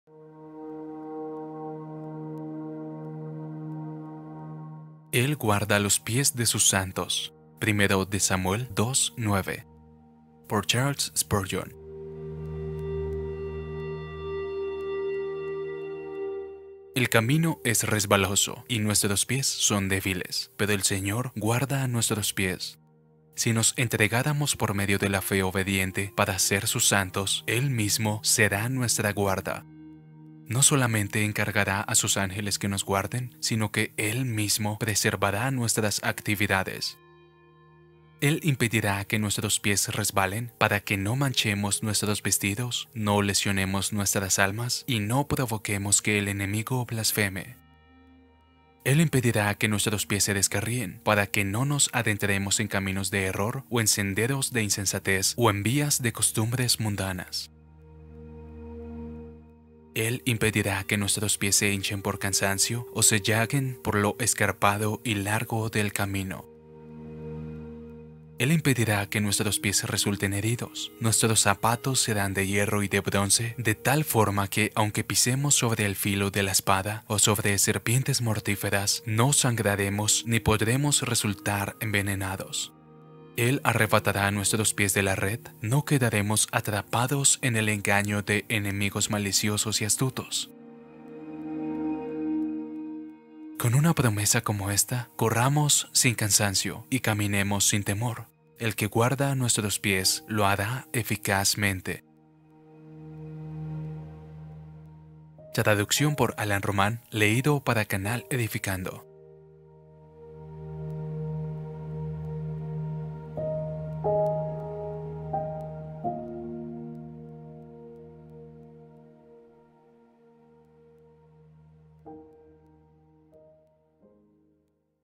Camina sin temor (Charles Spurgeon) - Meditación diaria
Créditos:
Chequera del Banco de la Fe
Canal Edificando